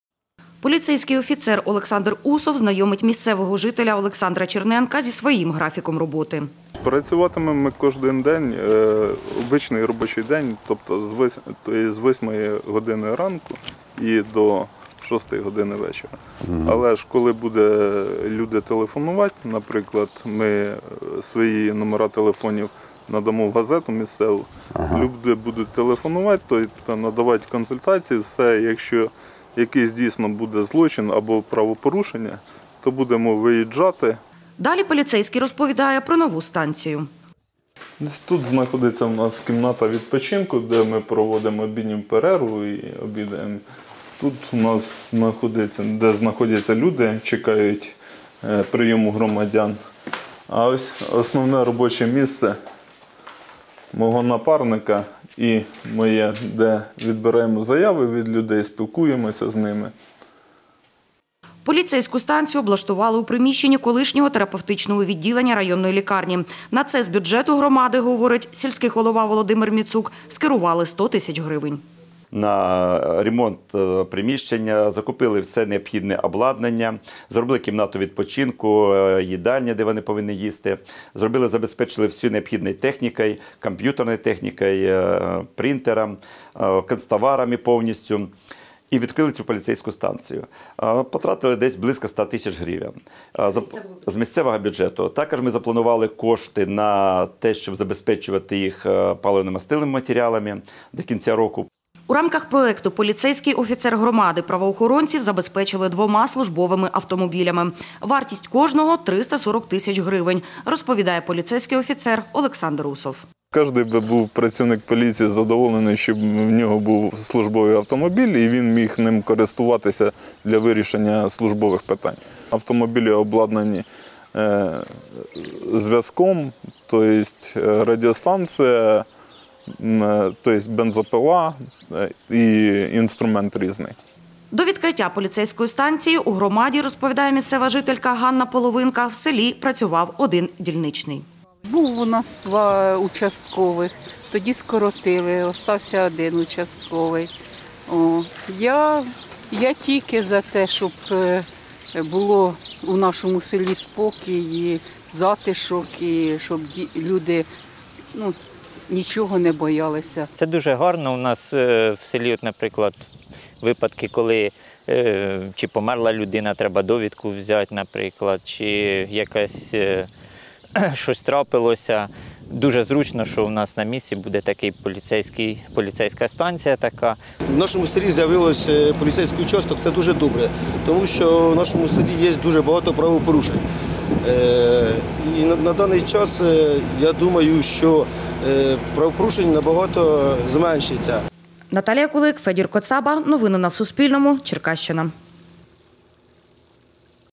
Поліцейська станція запрацювала у Білозірській ОТГ
Поліцейська станція запрацювала сьогодні у Білозірській об'єднаній громаді. Створили її у рамках проєкту "Поліцейський офіцер громади", розповів Суспільному голова громади Володимир Міцук. Йдеться про спільний проєкт Нацполіції та Міністерства юстиції США. Забезпечуватимуть правопорядок у селі Бiлозiр'я, селищах Баси та Iрдині два поліцейських офіцери. Більше Наталія Кулик розкаже.